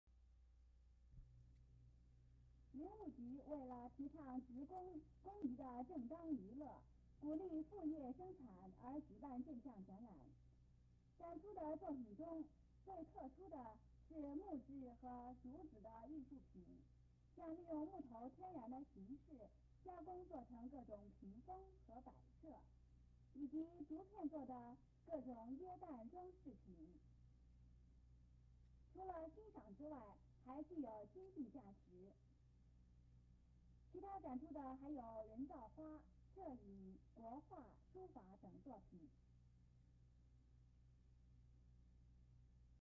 省林務局職工藝展.
(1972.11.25)省林務局為提升民眾正當娛樂而舉辦職工藝展.